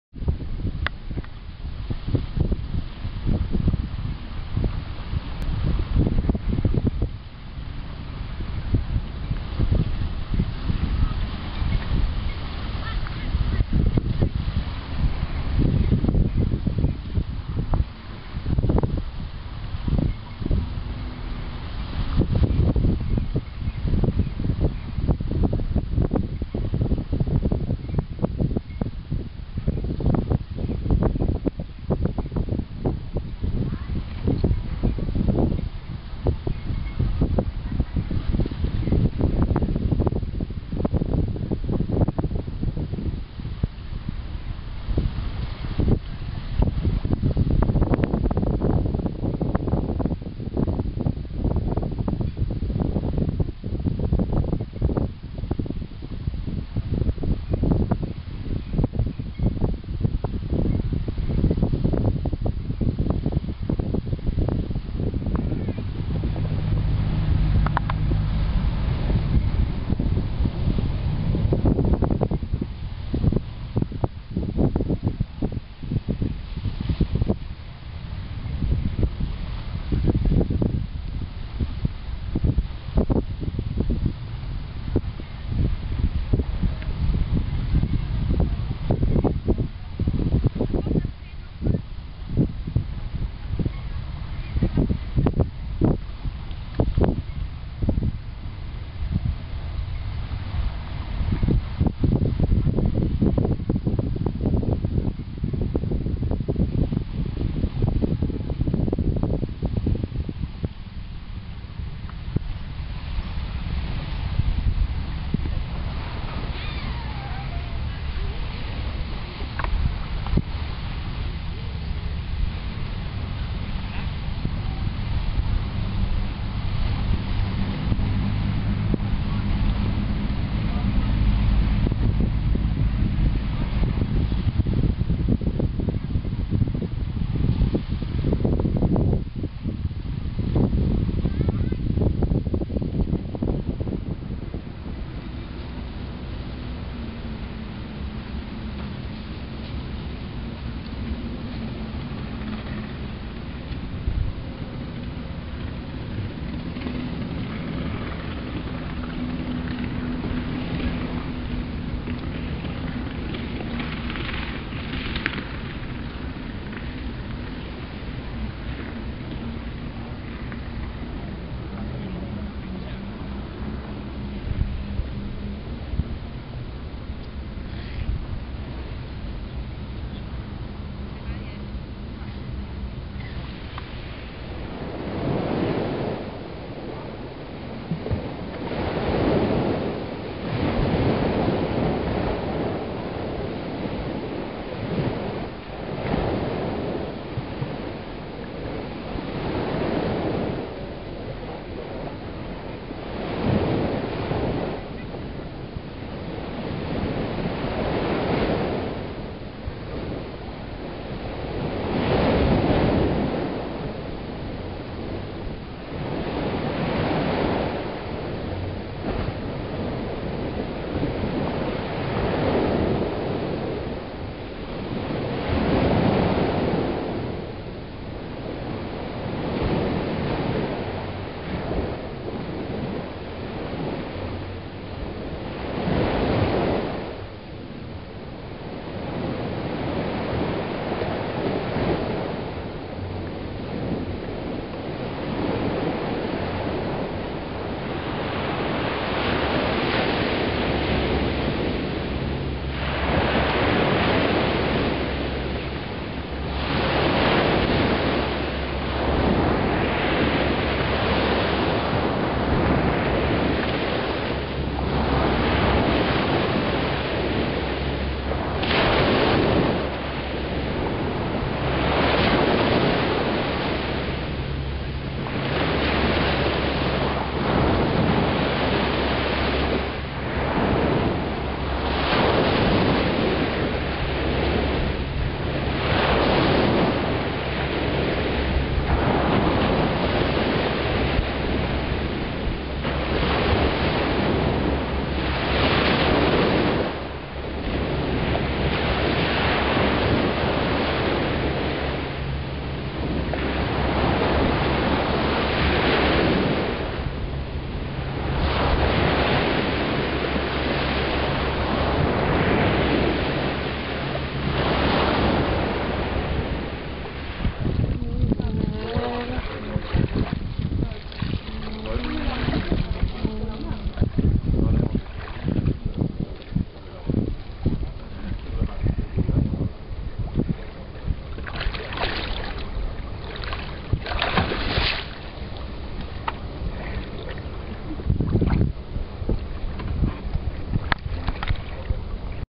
Thailand beaches
Waves crashing on the beach on various beaches in Thailand